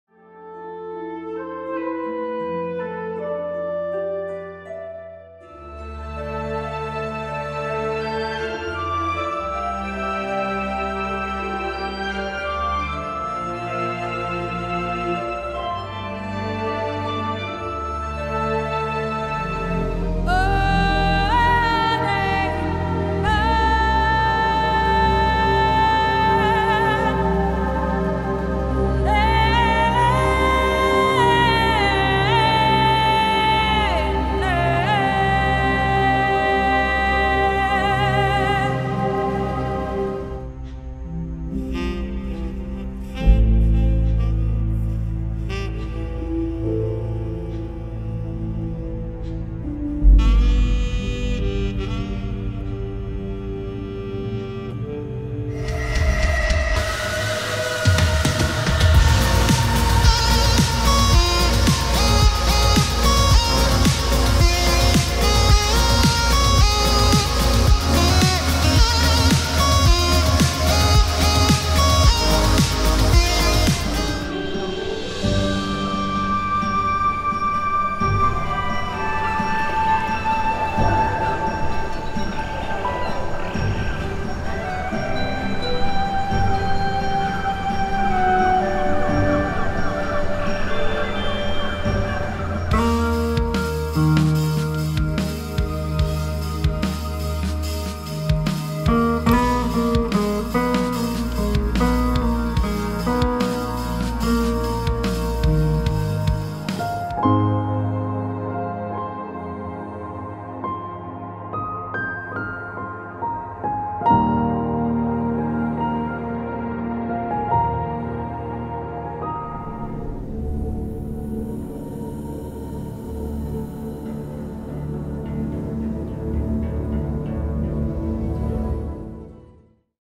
Nuno Coelho - Music Demo Reel
Small demo reel of several songs composed for games, animations and other projects